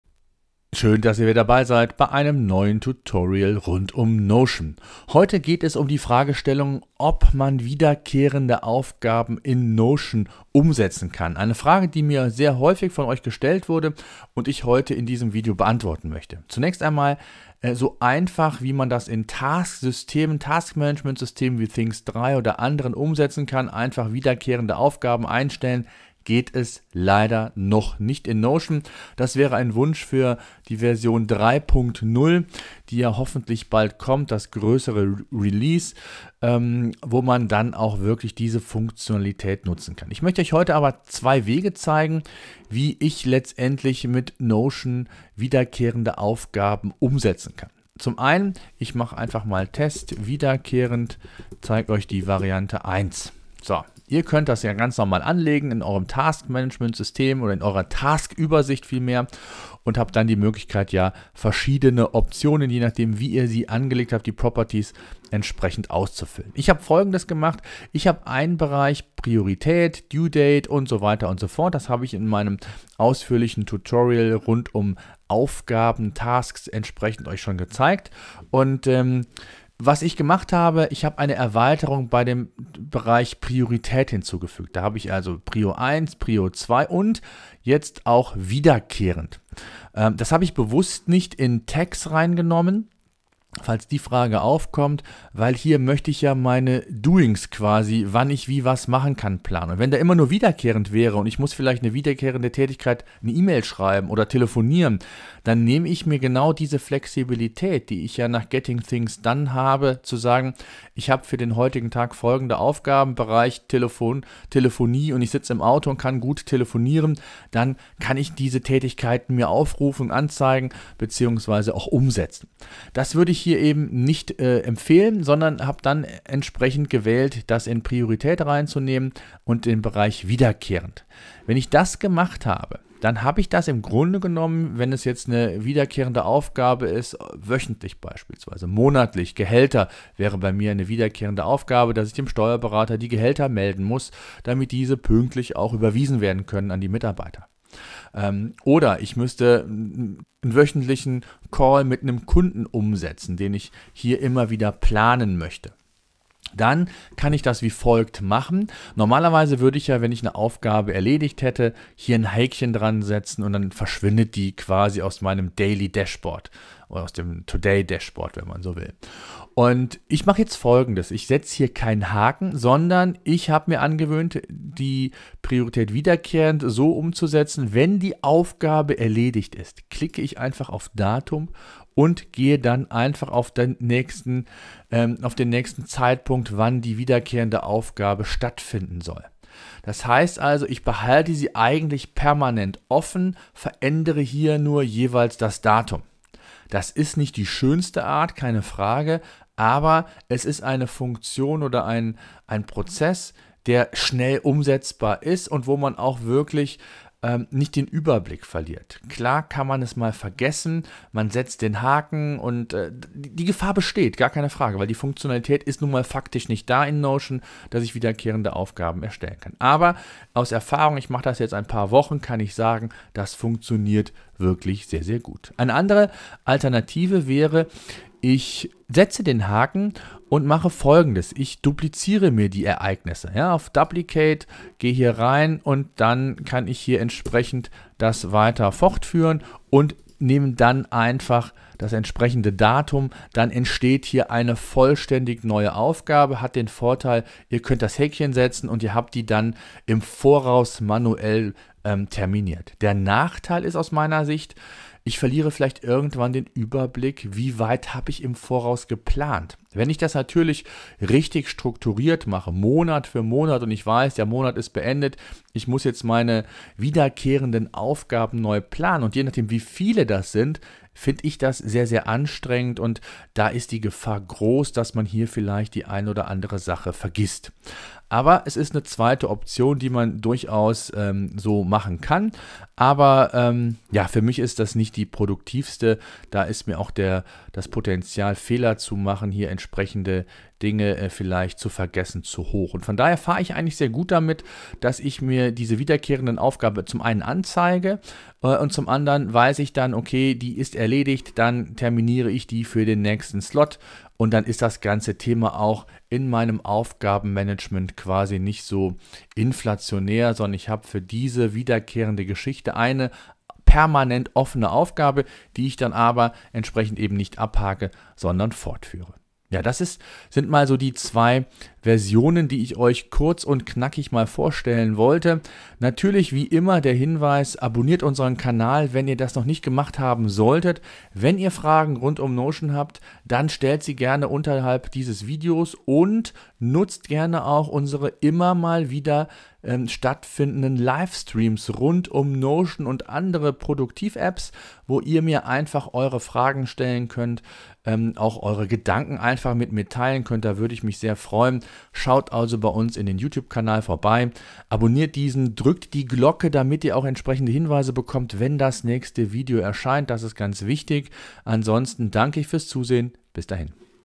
Wiederkehrende Aufgaben in Notion.so - so geht's!
In unserem heutigen Notion Tutorial zeige ich euch, wie man in Notion wiederkehrende Aufgaben umsetzen kann.